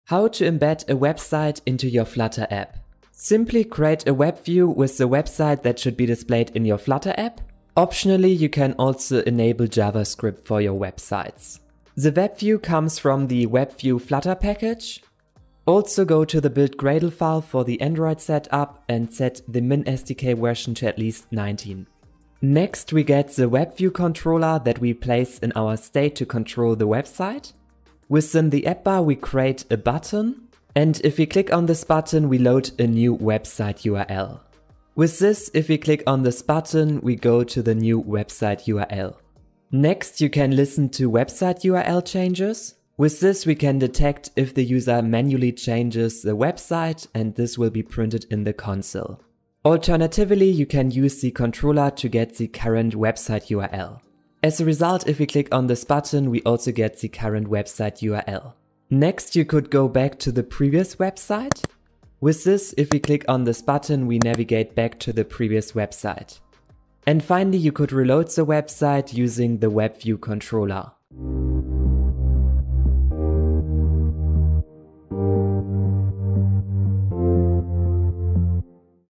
Flutter Tutorial - WebView [2022]
How to embed websites into your Flutter app using the WebView with javascript support in Flutter.

TIMELINE
0:00 WebView
0:22 WebView Controller